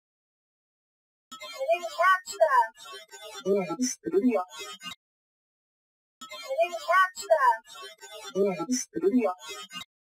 I didn't catch that Lewes Historical Society Investigation 2018
Spirit box response to investigator's statement, "I didn't catch that..." Female responds, "Erase the video." Recorded on November 8, 2018, at the Burton-Ingram House, Lewes Historical Society, Lewes, Delaware.